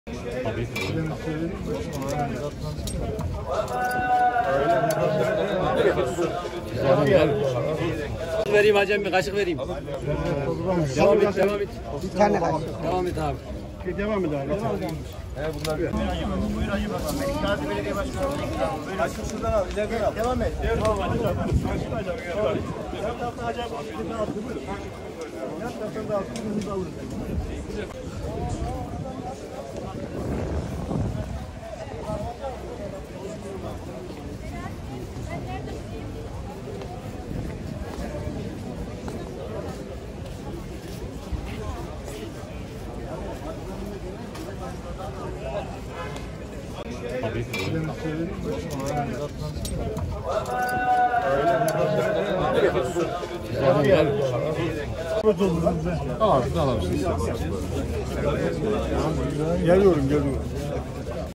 Kayseri'de Aşure ikramı
Melikgazi Belediye Başkanı Doç. Dr. Mustafa Palancıoğlu, paylaşmanın ve dayanışmanın simgesi ‘Aşure Günü’ dolayısıyla cuma namazı sonrası vatandaşlara aşure ikram ederek, günlerini tebrik etti.